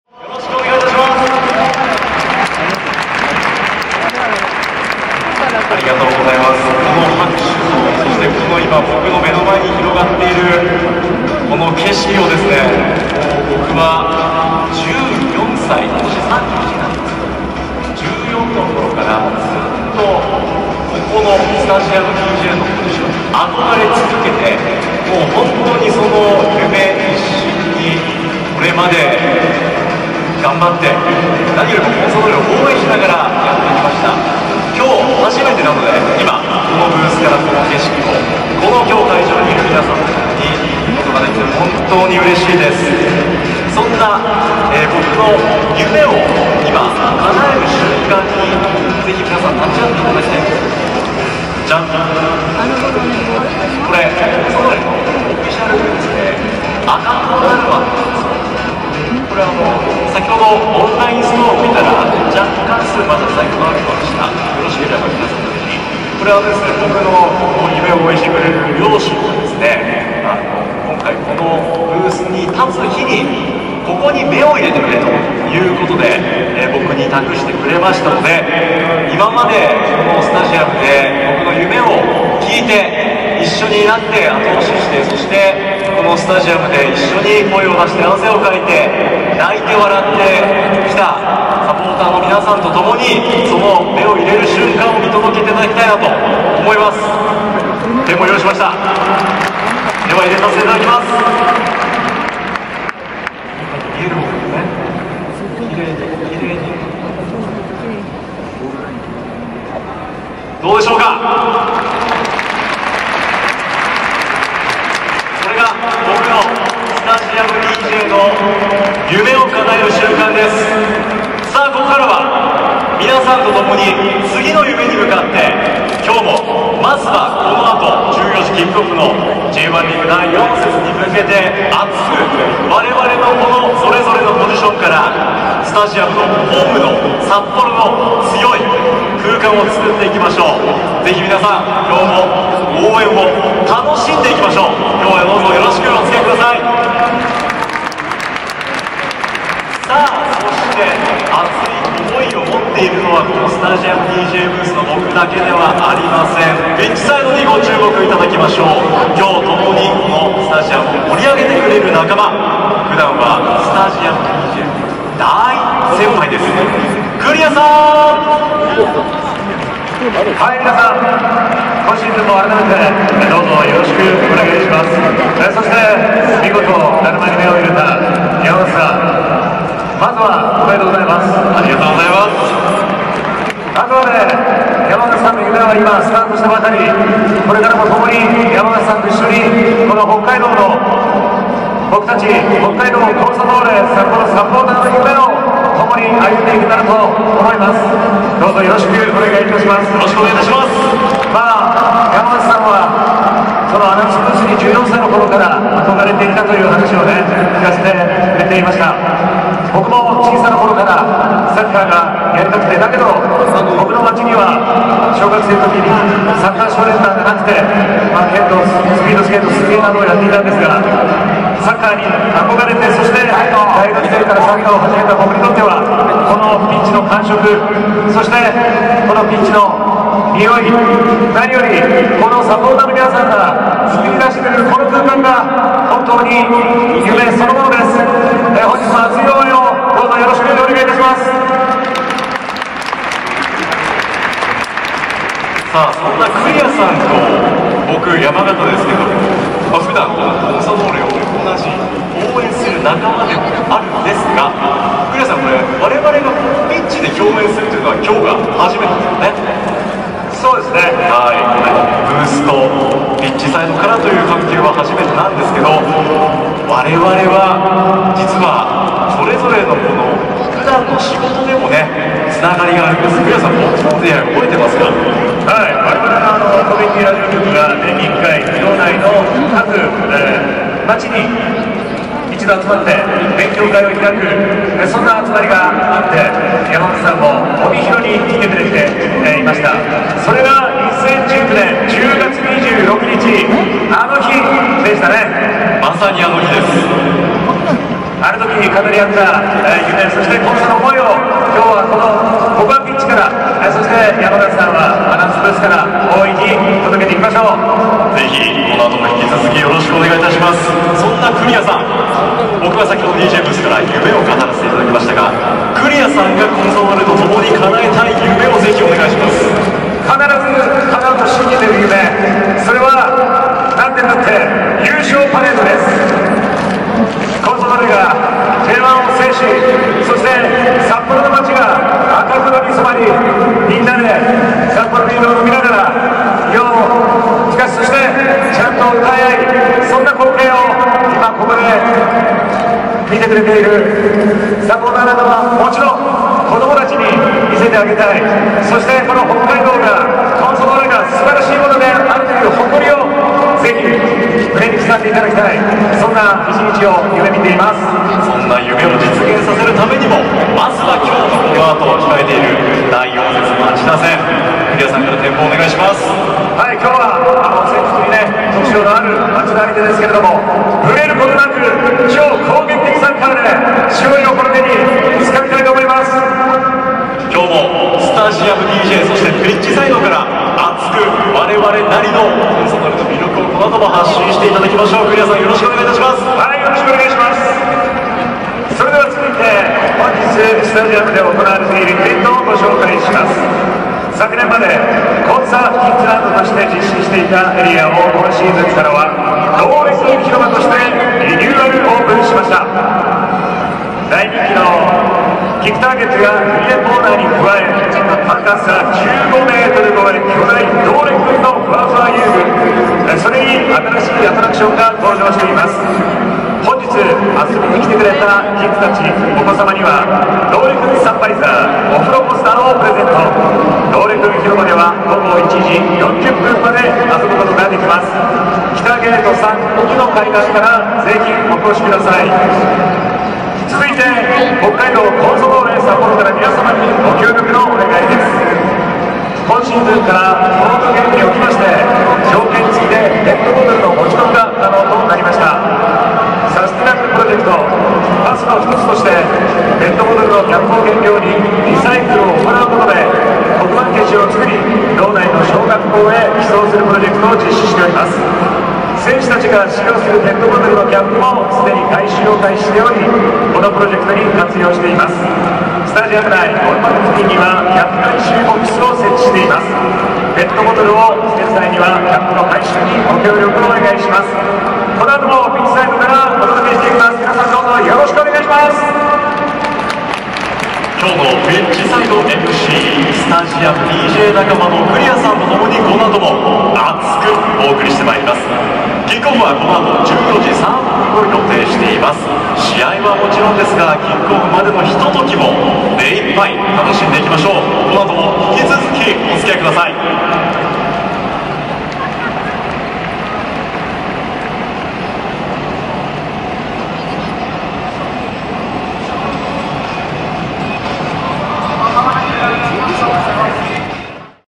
【札幌vs町田】祝 山形翼さん！スタジアムDJデビュー！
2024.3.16.北海道コンサドーレ札幌vs町田ゼルビア
山形翼さんが念願のスタジアムDJデビューを果たしました！
その熱い思いを語っています！